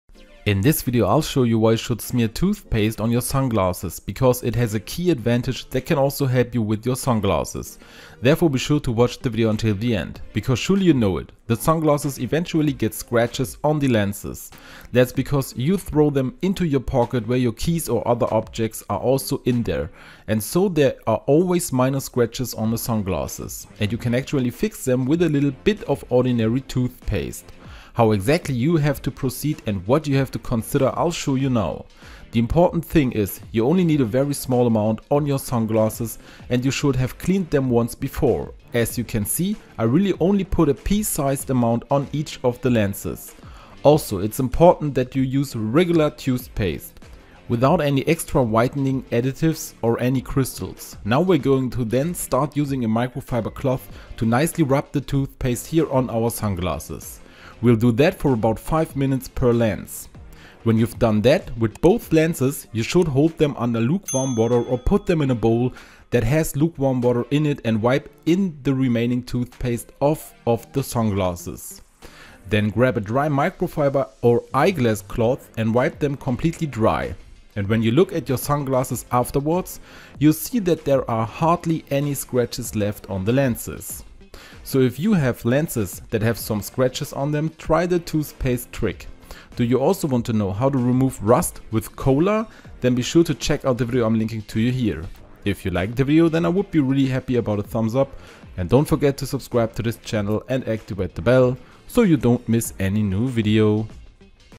Spread TOOTHPASTE on your SUNGLASSES and WATCH WHAT HAPPENS 💥 (surprisingly) 🤯
Have you ever spread toothpaste on your sunglasses? - Here I show you why you should!

Surely you also have some scratches on the lenses of your sunglasses. Over time, that's impossible to avoid. Especially if you put your sunglasses in your handbag, where other items are also inside.

But with a little toothpaste you get the scratches polished out of the lenses of the sunglasses again. At least as long as they are not so deep. To do this, you just need to put a small amount of toothpaste on the sunglasses and then rub with a cloth in circular motions.

Make sure that it is not toothpaste with large particles. These could scratch the lenses of your sunglasses even more. Then rinse off the toothpaste residue with water and minor scratches should be gone from the sunglasses.

Try this trick but also with you necessarily once.